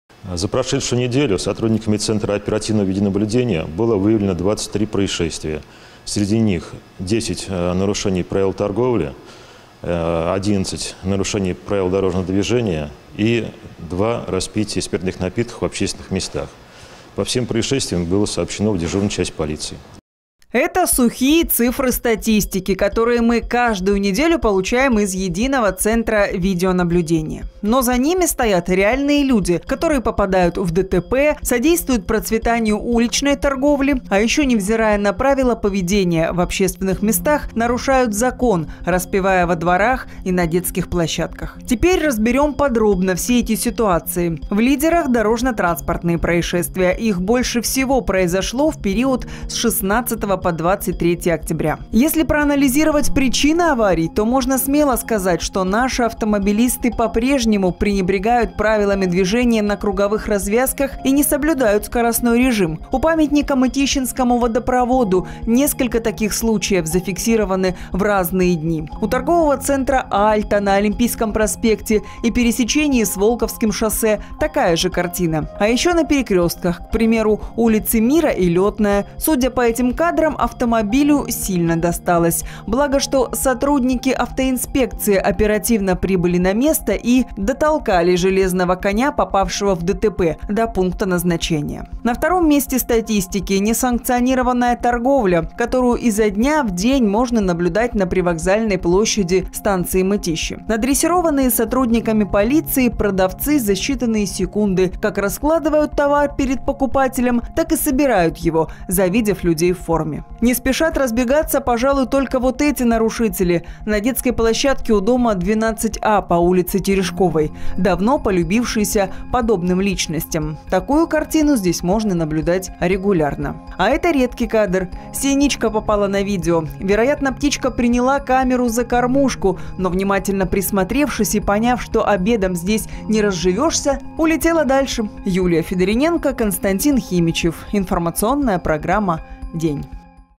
Сводка ЕЦВ
С  2015 года в Московской области внедряется самая масштабная программа - система видеонаблюдения «Безопасный регион». На данный момент в Подмосковье установлено почти 13 тысяч видеокамер. Средствами фиксации в обязательном порядке оборудованы места массового пребывания людей – парки, центральные площади, автодороги, въезды в города и микрорайоны, наиболее оживленные пешеходные переходы и перекрестки с интенсивным движением.